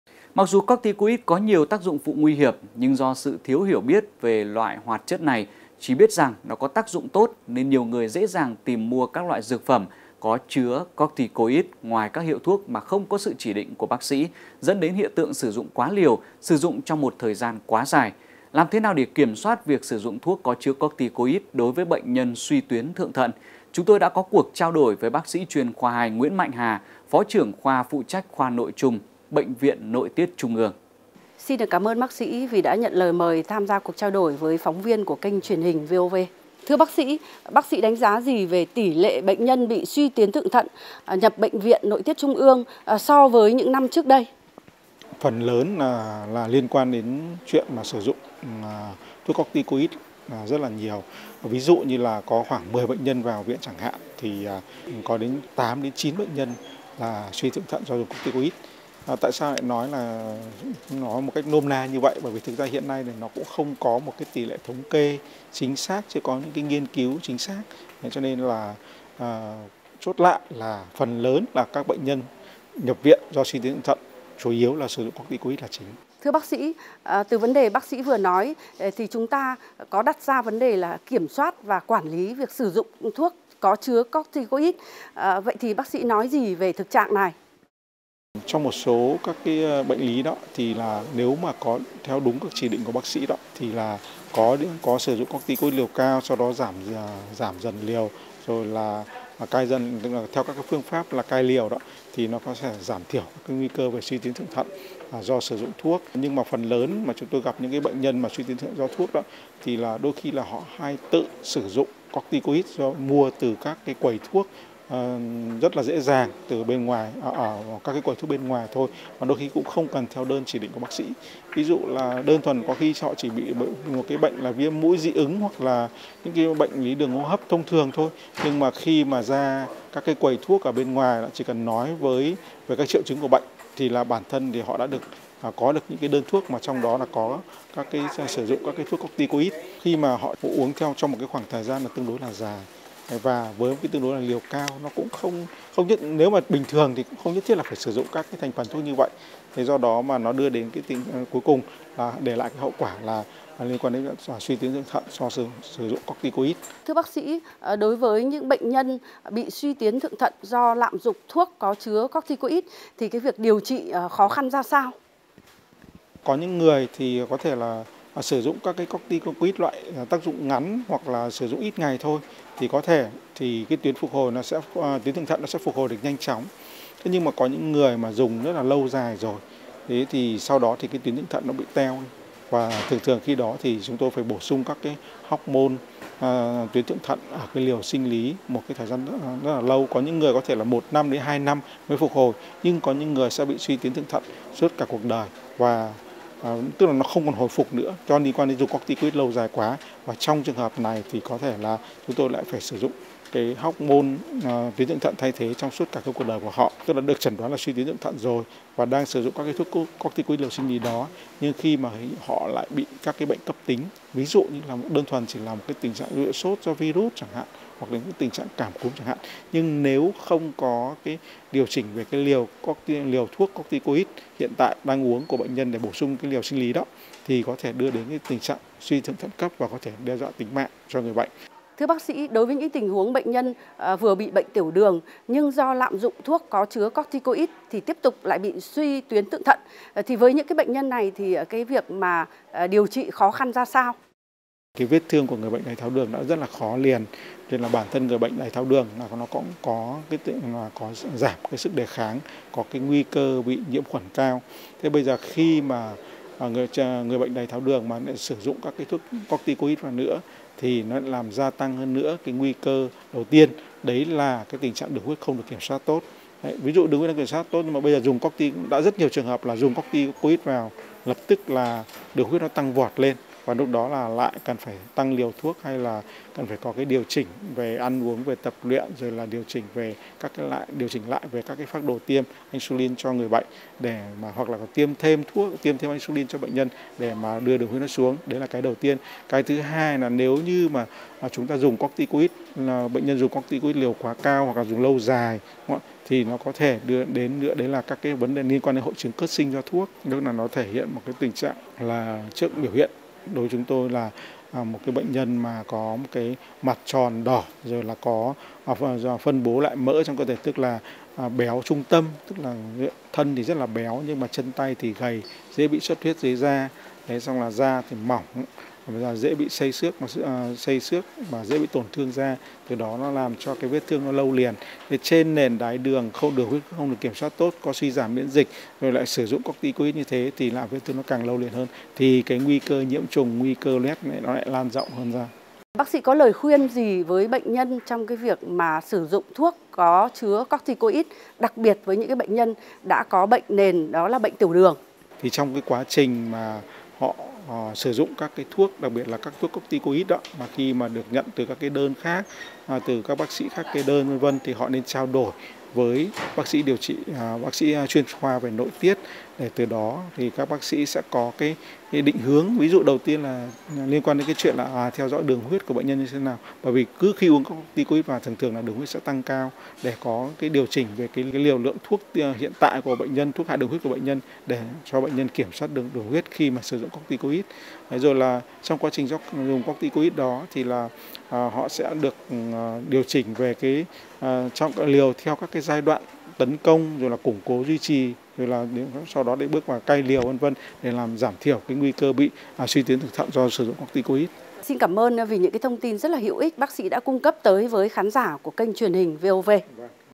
Suy tuyến thượng thận - Làm thế nào để kiểm soát việc sử dụng thuốc có chứa corticoid?
Mặc dù corticoid có nhiều tác dụng phụ nguy hiểm, nhưng do sự thiếu hiểu biết về loại hoạt chất này, chỉ biết rằng nó có tác dụng tốt, nên nhiều người dễ dàng tìm mua các loại dược phẩm có chứa corticoid ngoài các hiệu thuốc mà không có sự chỉ định của bác sĩ, dẫn đến hiện tượng sử dụng quá liều, sử dụng trong một thời gian quá dài.

Làm thế nào để kiểm soát việc sử dụng thuốc có chứa corticoid đối với bệnh nhân suy tuyến thượng thận, chúng tôi có cuộc trao đổi với  BSCKII Nguyễn Mạnh Hà - Phó Trưởng khoa phụ trách Khoa Nội chung - Bệnh viện Nội tiết Trung ương.